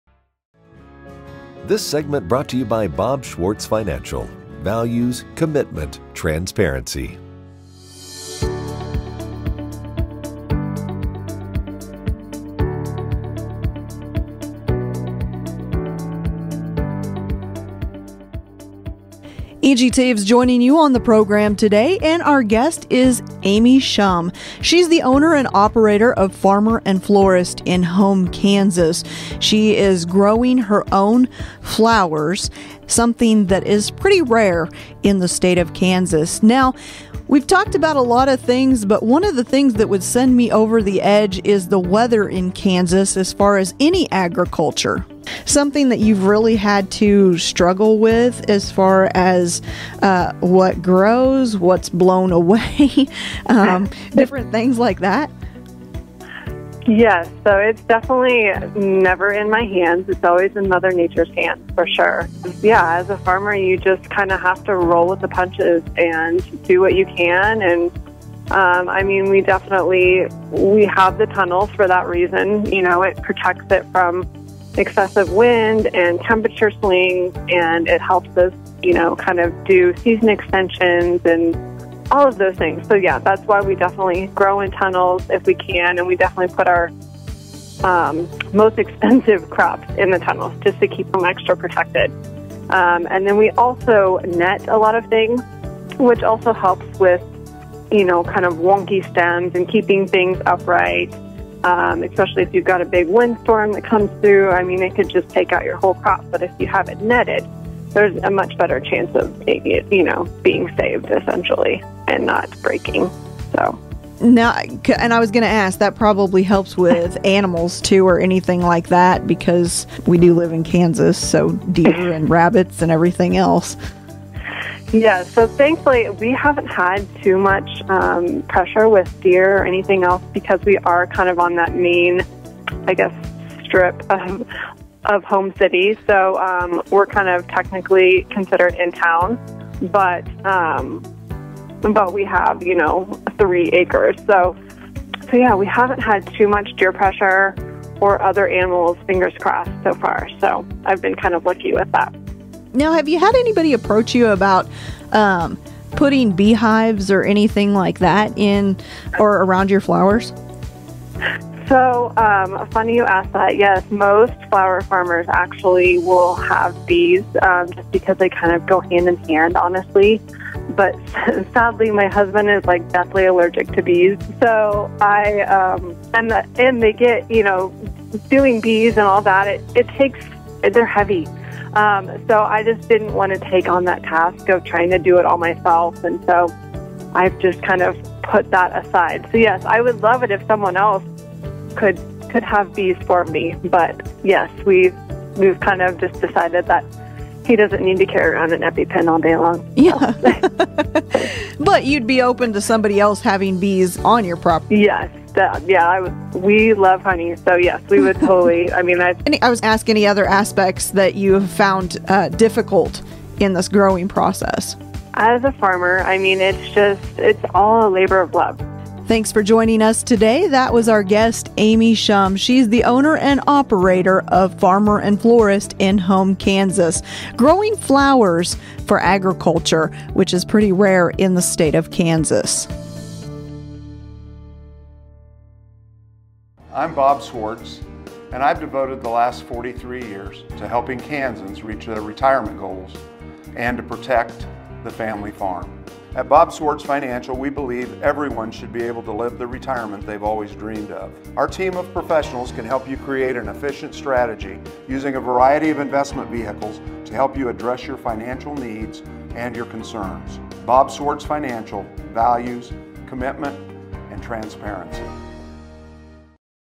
OFP Farmer and Florist Amy Shum
Amy Shum is the owner and operator of Farmer and Florist in Home, Kansas. She is growing her own flowers something that is pretty rare in the state of Kansas. Now we've talked about a lot of things but one of the things that would send me over the edge is the weather in Kansas as far as any agriculture something that you've really had to struggle with as far as what grows, what's blown away different things like that.

- Yes, so it's definitely never in my hands. It's always in mother nature's hands for sure. Yeah, as a farmer, you just kinda have to roll with the punches and do what you can. And I mean, we definitely, we have the tunnel for that reason. You know, it protects it from excessive wind and temperature sling and it helps us kind of do season extensions and all of those things. So yeah, that's why we definitely grow in tunnels if we can. And we definitely put our most expensive crops in the tunnels just to keep them extra protected. And then we also net a lot of things which also helps with kind of wonky stems and keeping things upright especially if you've got a big wind storm that comes through I mean, I could just take out your whole crop, but if you have it netted, there's a much better chance of being saved essentially and not breaking.

- Now, and I was gonna ask that probably helps with animals too or anything like that because we do live in Kansas so deer and rabbits and everything else.

- Yes, so thankfully we haven't had too much pressure with deer or anything else because we are kind of on that mean, I guess, strip of Home cities. So we're kind of technically considered in town, but we have, you know, three acres. So, yeah, we haven't had too much deer pressure or other animals fingers crossed so far. So I've been kind of lucky with that.

- Now, have you had anybody approach you about putting beehives or anything like that in or around your flowers?

- So funny you asked that yes most flower farmers actually will have bees just because they kind of go hand in hand honestly but sadly my husband is like deathly allergic to bees, and doing bees and all that, they're heavy. So I just didn't wanna take on that task of trying to do it all myself. And so I've just kind of put that aside. So yes, I would love it if someone else could have bees for me, but yes, we've kind of just decided that he doesn't need to carry on an EpiPen all day long.

- But you'd be open to somebody else having bees on your property?

- Yes we love honey so yes, we would totally. I mean that's--

- Amy I was asking any other aspects that you have found difficult in this growing process.

- As a farmer, it's all a labor of love.

- Thanks for joining us today. That was our guest Amy Shum. She's the owner and operator of Farmer and Florist in Home, Kansas growing flowers for agriculture which is pretty rare in the state of Kansas.